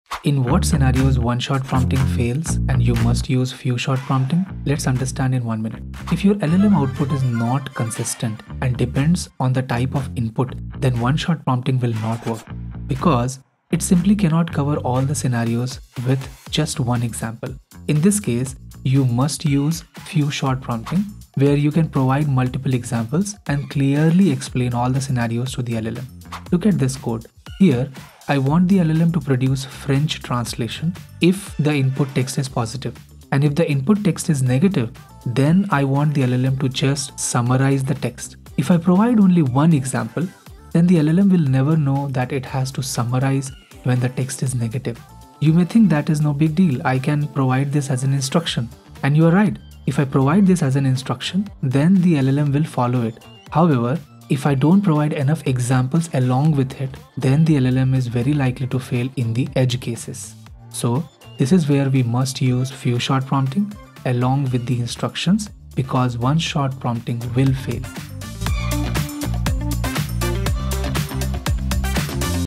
Is Few Shot Prompting better than One Shot?
When exactly one shot prompting fails and why should I use Few shot prompting?

The code that I used in the Video:
Code Begins
import os
from openai import OpenAI

# Set your OpenAI API key (replace with your OpenAI Key)
os.environ["OPENAI_API_KEY"] = "sk-proj-1Zxa0vihj231JASXks0qT3BlbkFJmvmIDwd9Y1wjWt1Wge7V"

# Initialize OpenAI_Client
OpenAI_Client = OpenAI()

def ask_GPT_model(prompt: str):

    """Send a prompt to the GPT model and return the response."""
    response = OpenAI_Client.chat.completions.create(
                model="gpt-4o-mini",  # or "gpt-4", "gpt-3.5-turbo"
                messages=[{"role": "user", "content": prompt}],
                temperature=0  # make it deterministic

    return response.choices[0].message.content

Few shot Prompt example
# We want French translation
# if the input text is positive
# and if the input text is negative
# then summarize it

# If we dont provide multiple examples,
# then the LLM will not be able to figure out
# the hidden rules

few_shot_prompt = """
you are an intelligent agent and you must
translate the input text to French
if it is positive and summarize it if the
input text is negative

Examples:

Input: I just got a promotion! I'm so thrilled!
Output:
  "mode": "translation",
  "result": "Je viens d'obtenir une promotion ! Je suis tellement ravi !"

Input: I failed my exam and feel terrible.
Output:
  "mode": "summary",
  "result": "Expresses disappointment after failing an exam."

Input: My team won the championship!
Output:
  "mode": "translation",
  "result": "Mon équipe a remporté le championnat !"

Input: I feel lonely after my friend moved away.

print("Few-shot response:")
print(ask_GPT_model(few_shot_prompt))

Code Ends

In this series of shorts I am covering the absolute basics you must know before attending a Data Science interview.